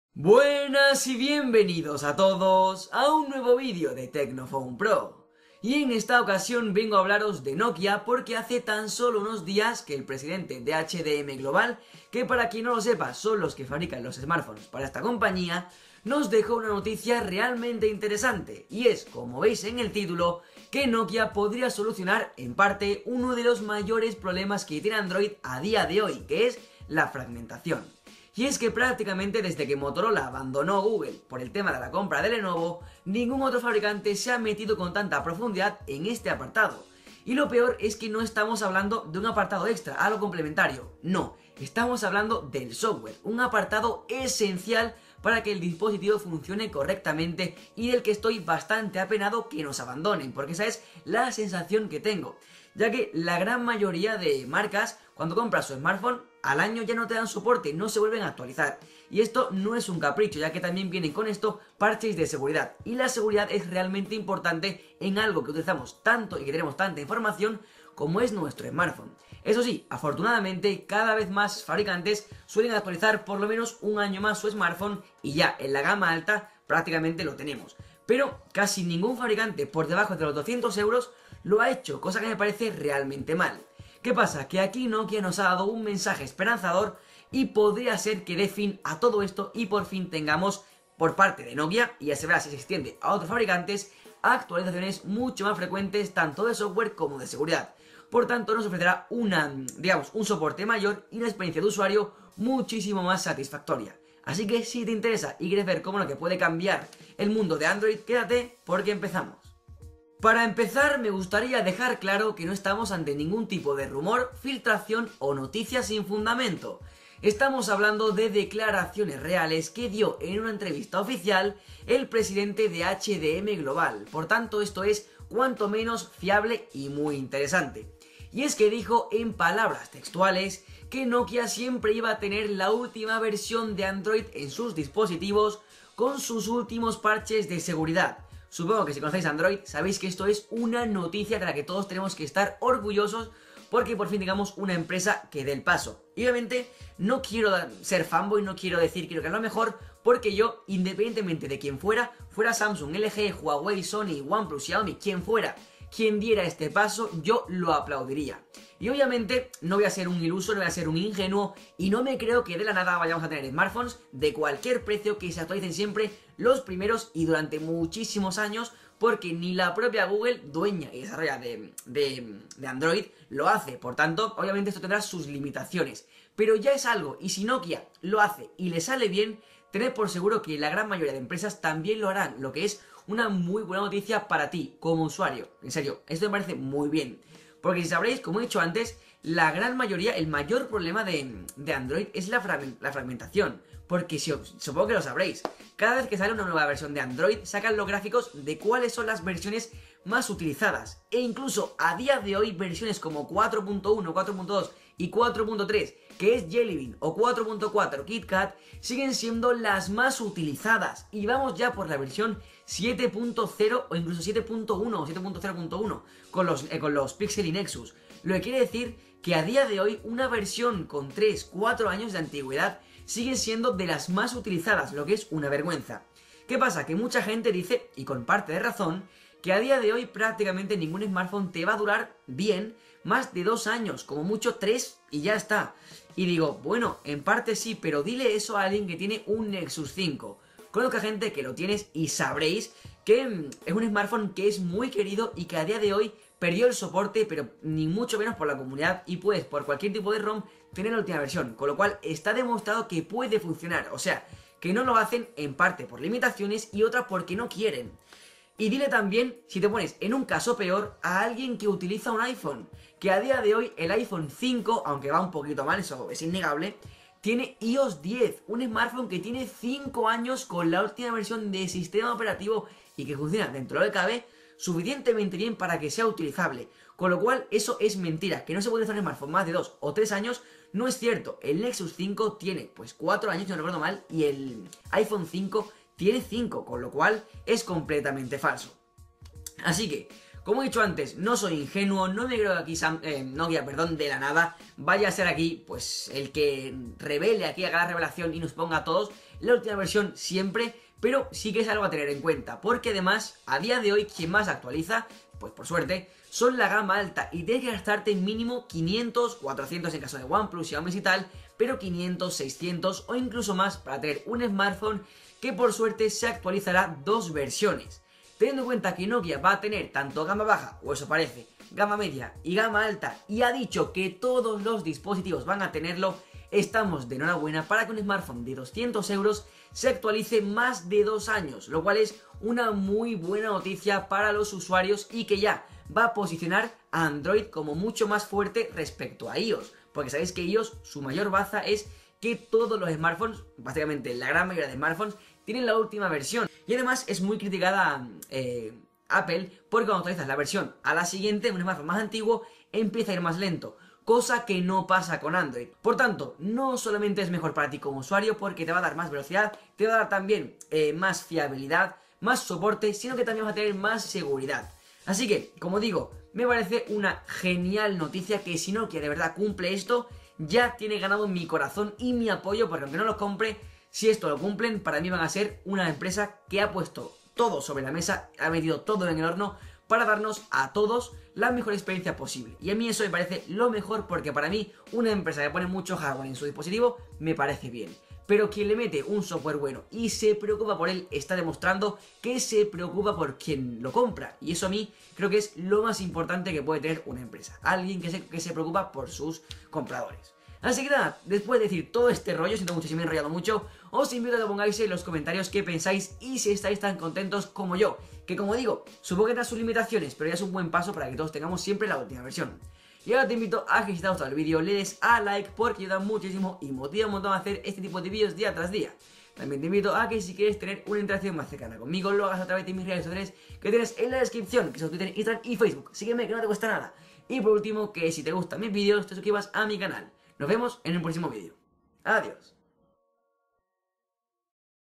Nokia Solucionará el Mayor Problema de Android
Nuevo vídeo en el que hablo de los próximos smartphones de Nokia, que podrían traer la mejor característica que puede traer un smartphone Android, ser actualizado tanto y tan rápido como los nuevos Google Pixel y Nexus, para acabar así con el peor problema de Android, la fragmentación

Puedes hacerme preguntas por twitter mencionando a @TecnoPhonePro con el Hashtag